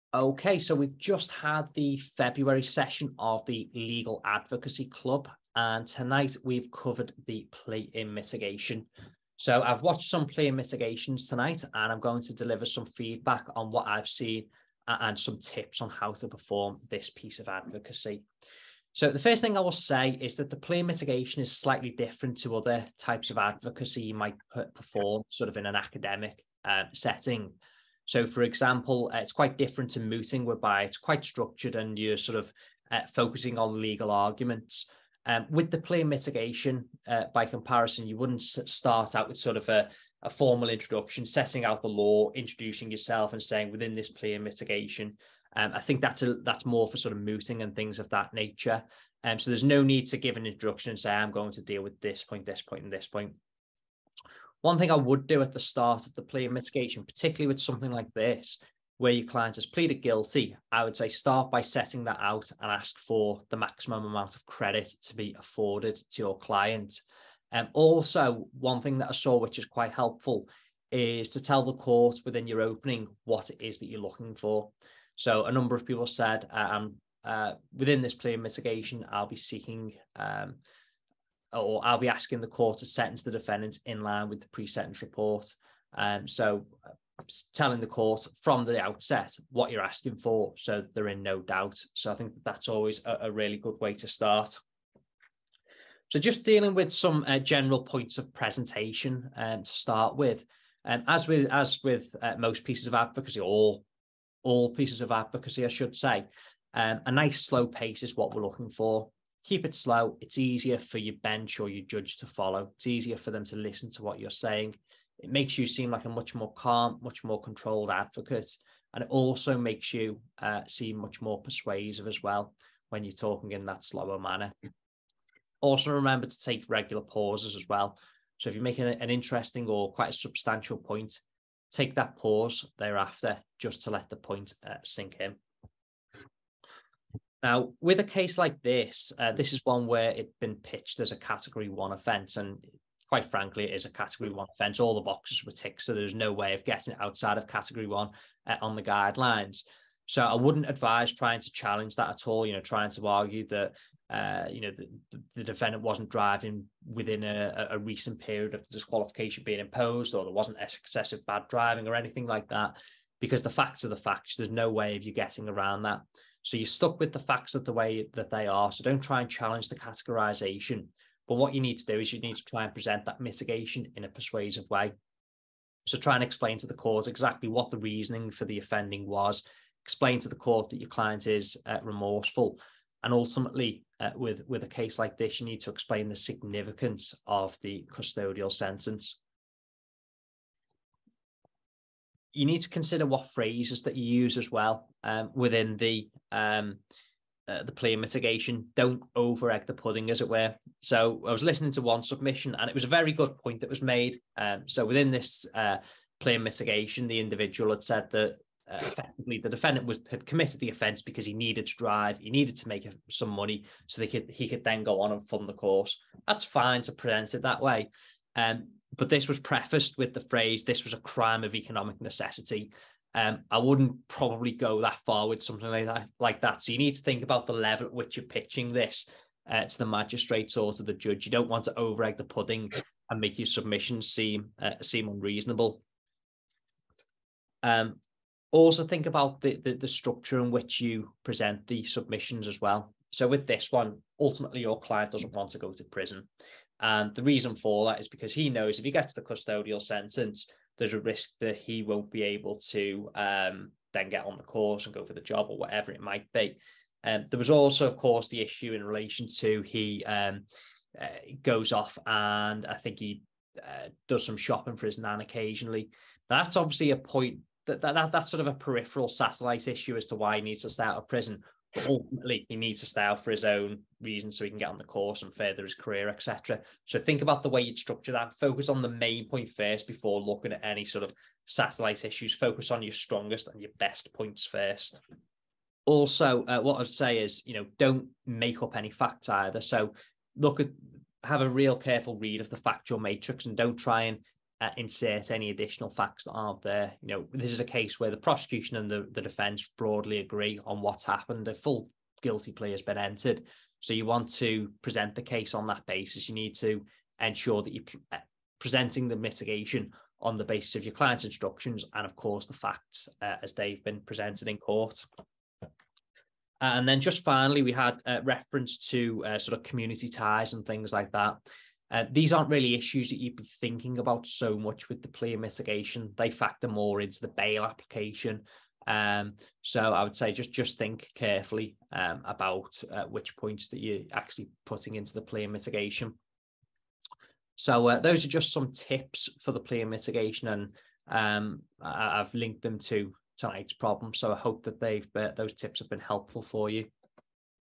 John Dove's Plea in Mitigation Tips
In this video that was recorded at the February 2023 Legal Advocacy Club session, Speed Mooting founder, John Dove, shares his top tips for the plea in mitigation. In the video, John covers the following points:

• The difference in the plea in mitigation and other forms of advocacy such as mooting
• General presentation tips
• Good phrases to use
• How to make points effectively
• being sensible in relation to the categorisation of an offence on the sentencing guidelines
• How to be persuasive

For more information on the Advocacy Club,